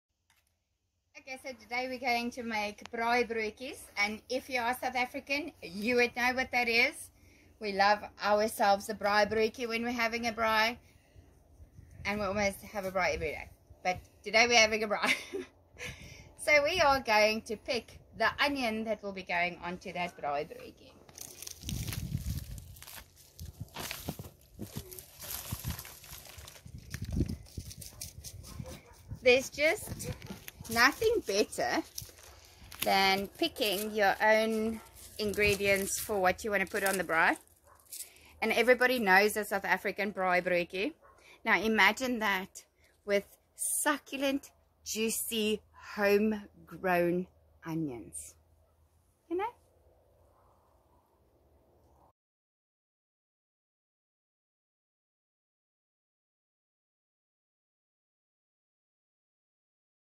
Harvesting an onion to make a delicious South African Braai Broodjie
Delicious homegrown onions, tomato and cheese on a sandwich.  Toasted slowly over medium hot coals.  Yummy!